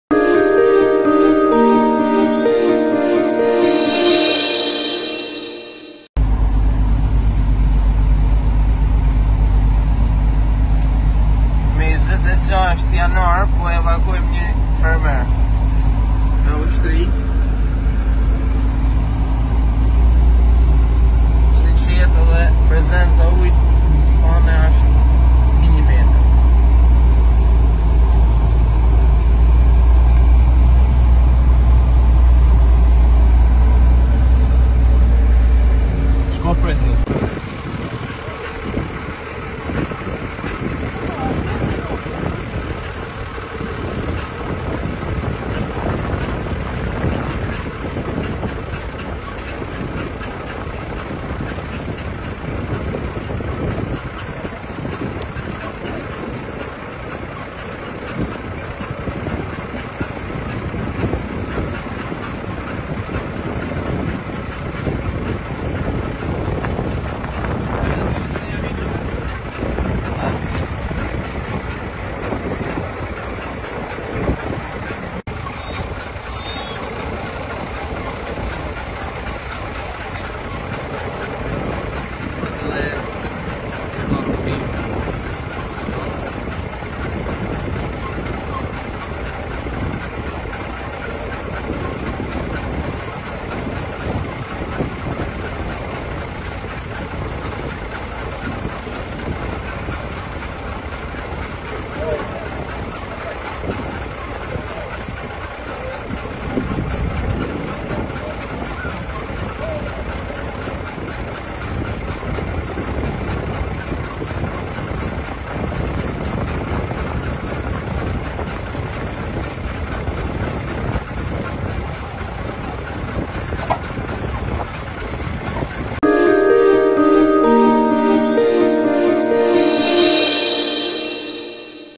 ShkodraWeb I Fillon evakuimi i gjësë së gjallë në fshatin Darragjat të Bashkisë së Shkodrës
Fillon evakuimi i gjësë së gjallë në fshatin Darragjat të Bashkisë së Shkodrës